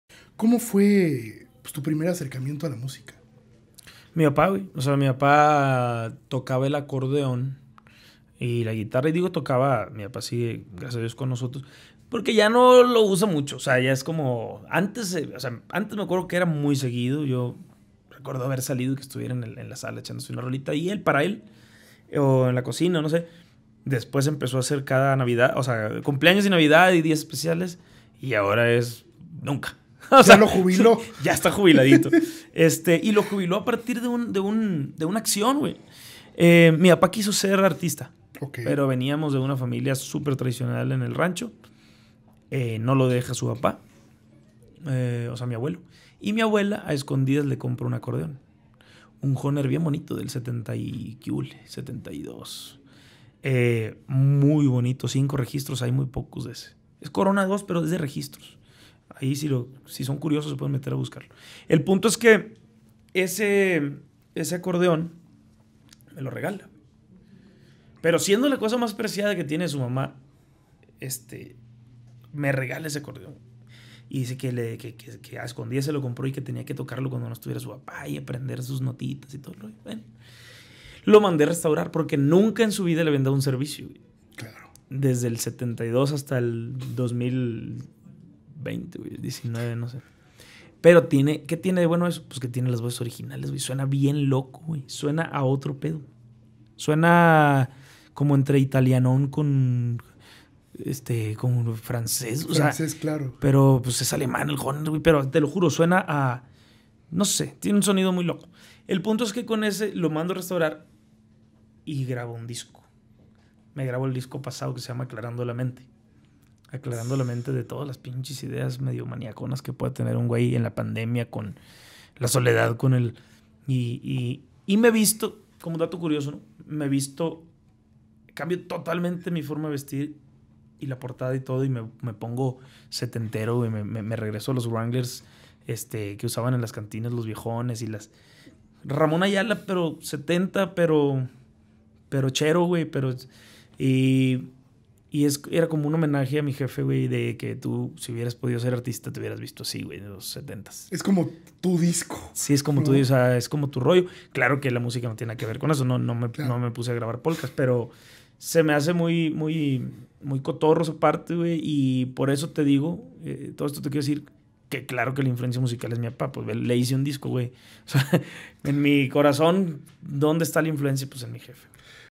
Mi primer acercamiento a la música | Charla con Joss Favela
🚨 Episodio completo 🚨